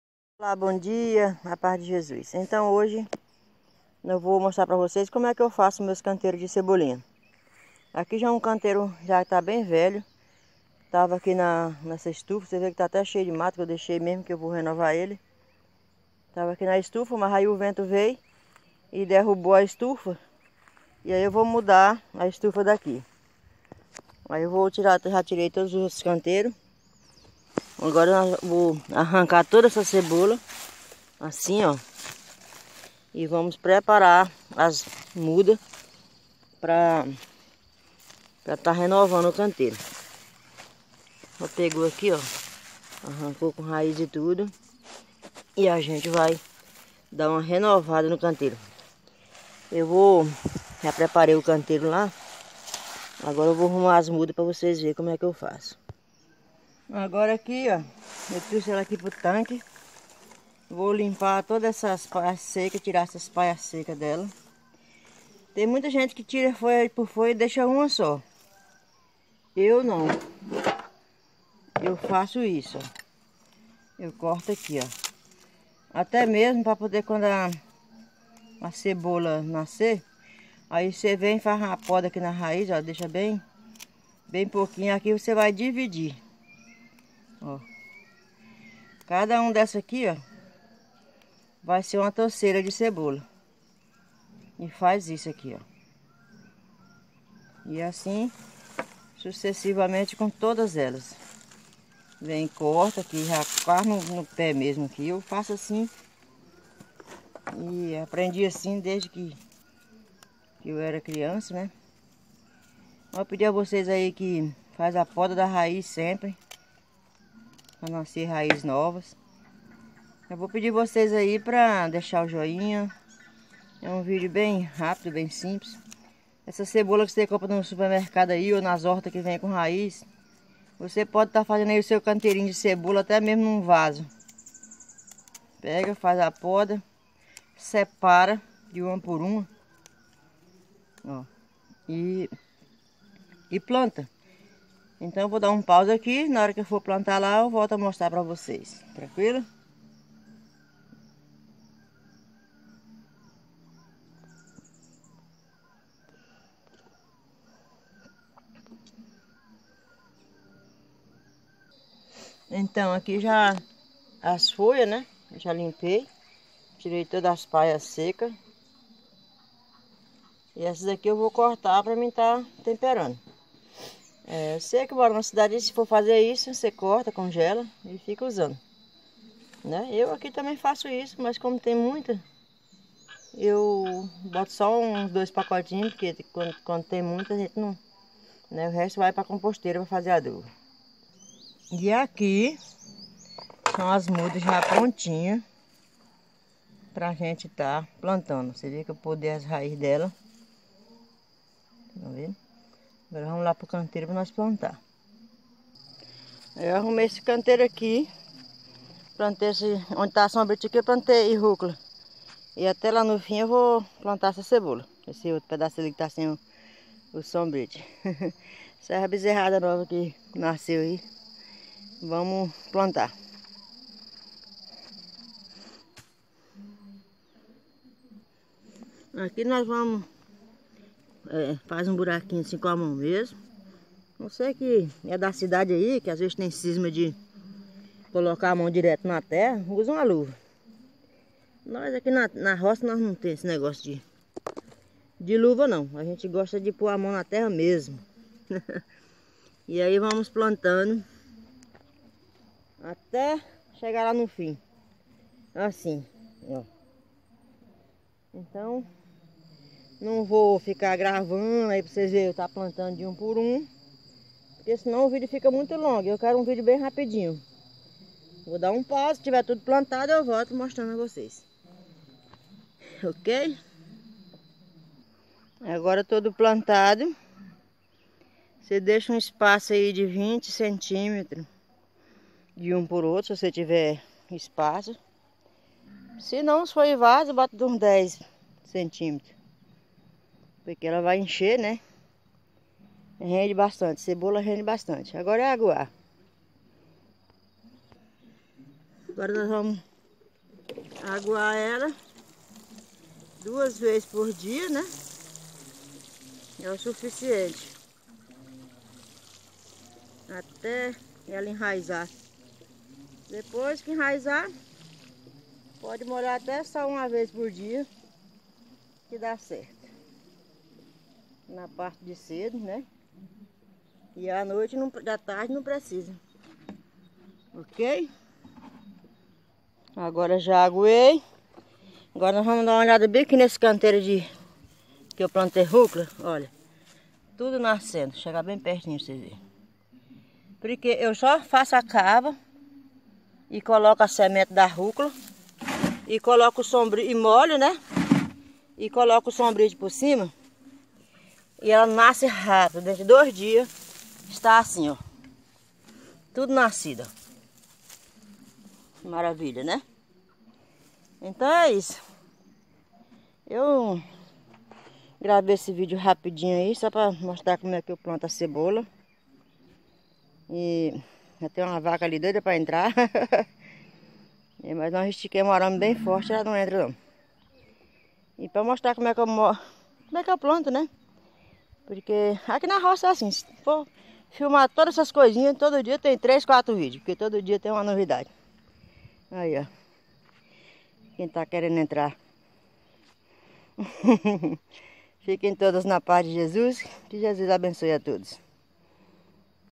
Como plantar cebolinha no canteiro
Meu nome.Ana Maria
Como fazer um canteiro de cebolinha pela muda
Jesus Cristo abençoe você